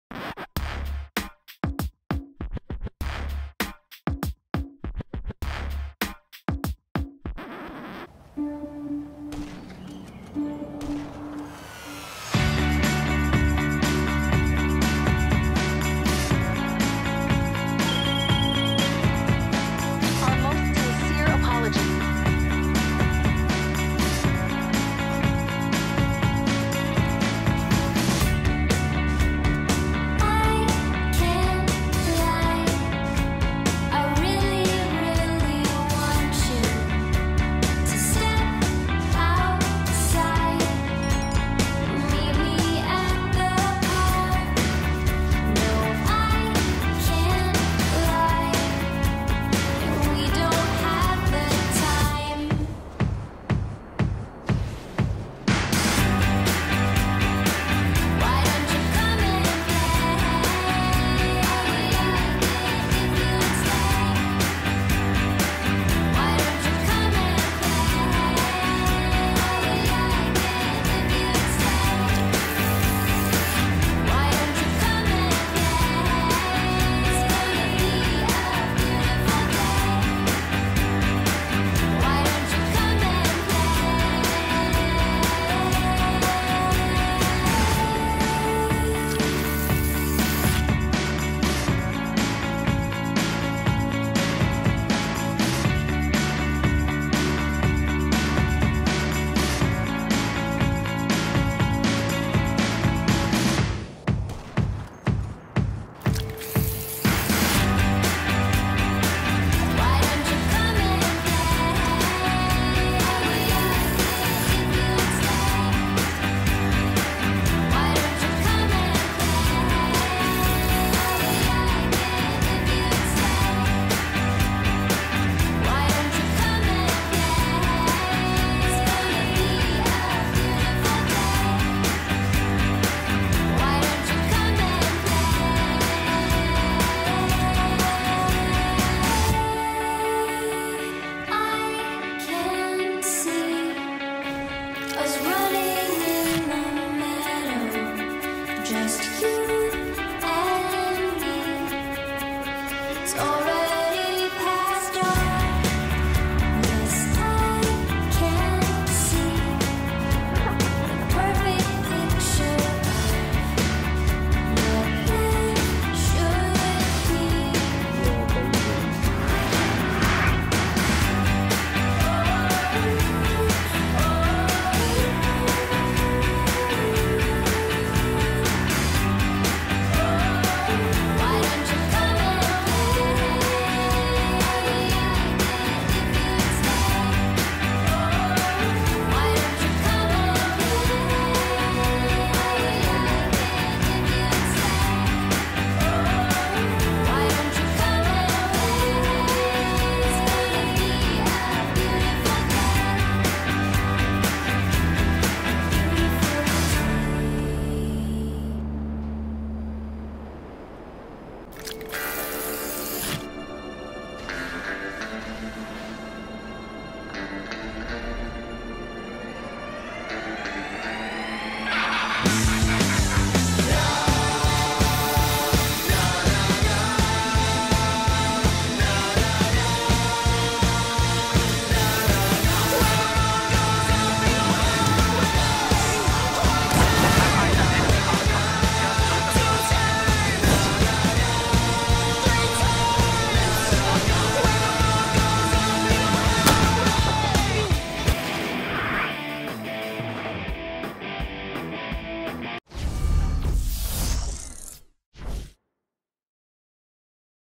PPR projects first gaming montage
Some clips of my game play over the last month or so.